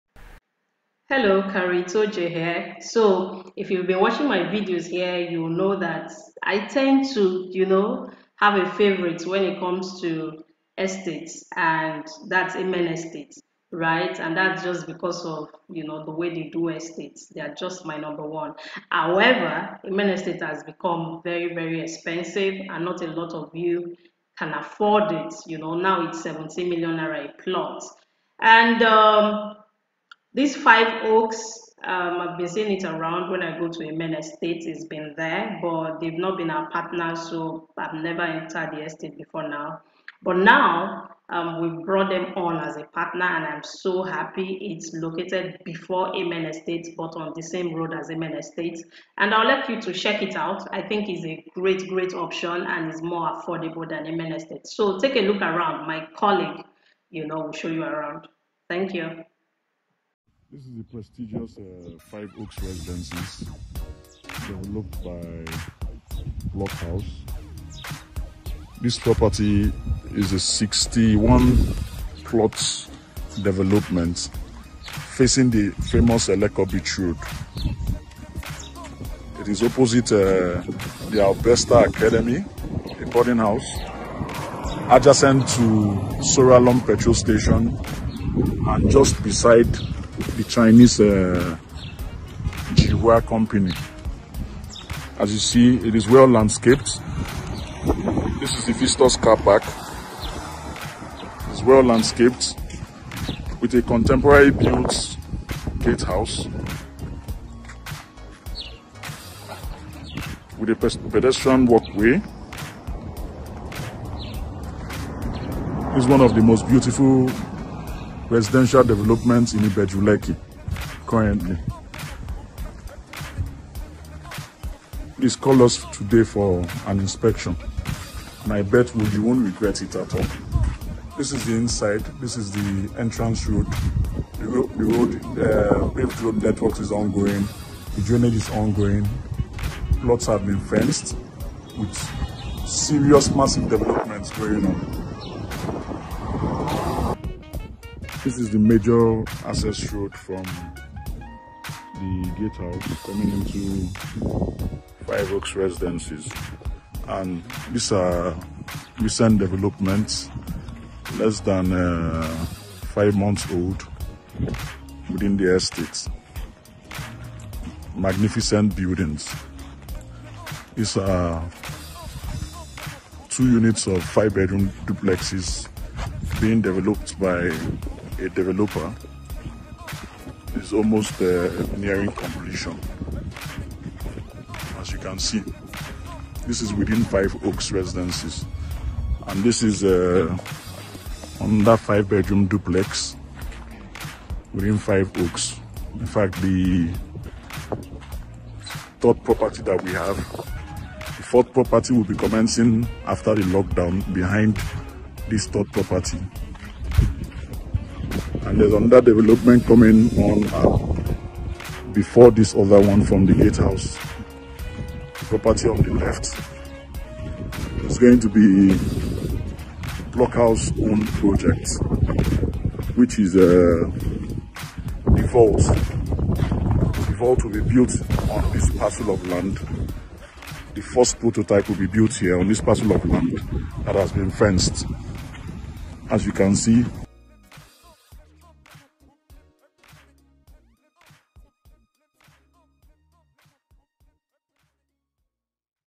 Five Oaks Residences Eleko Ibeju Lekki Before Amen Estate
If you're looking for an organized estate that is affordable or you like Amen estate but can't afford it, this is a great alternative.

The land is completely dry with great title and in a great location. And it's ready to build. If you're looking for an affordable land where you can start building immediately or a land to offer short term returns then Five Oaks Residences is a great one.